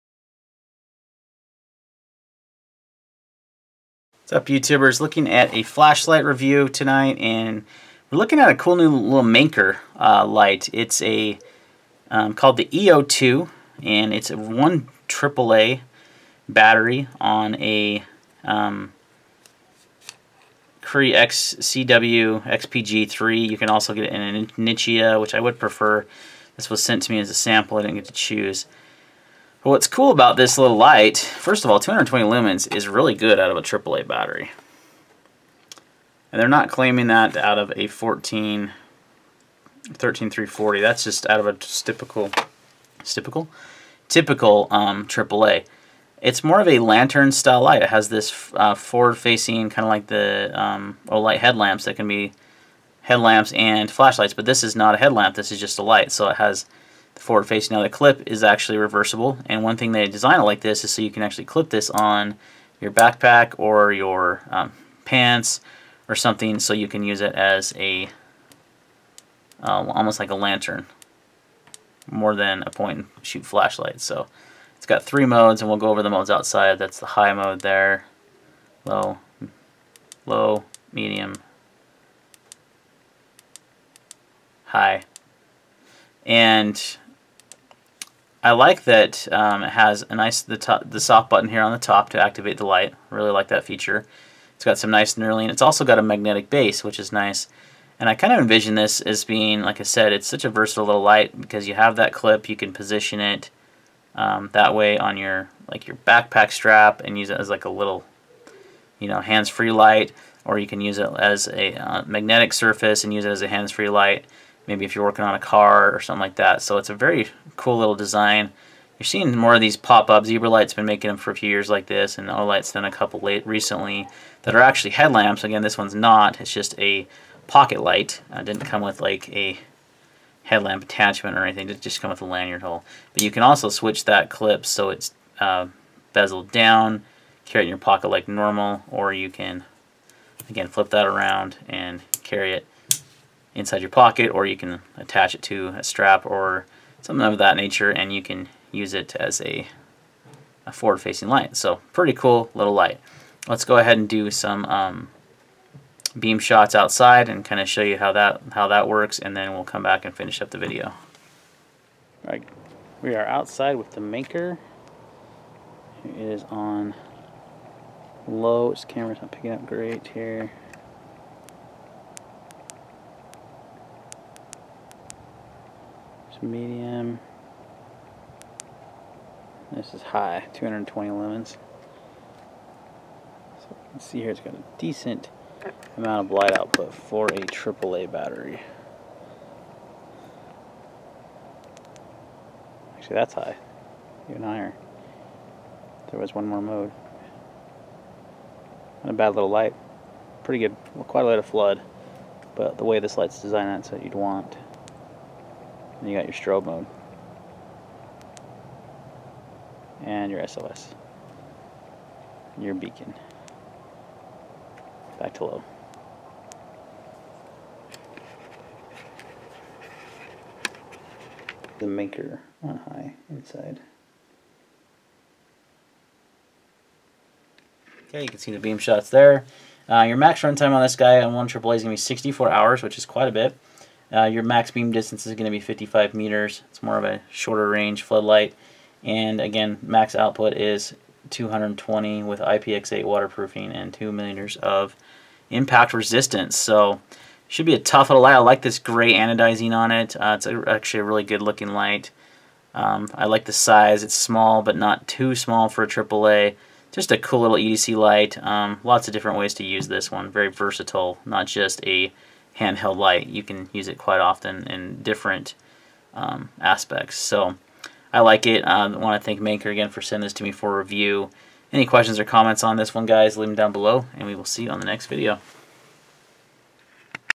Manker E02 Flashlight Review
In this video I review the Manker E02 AAA light. This little guy packs some great features and a good output for a AAA light.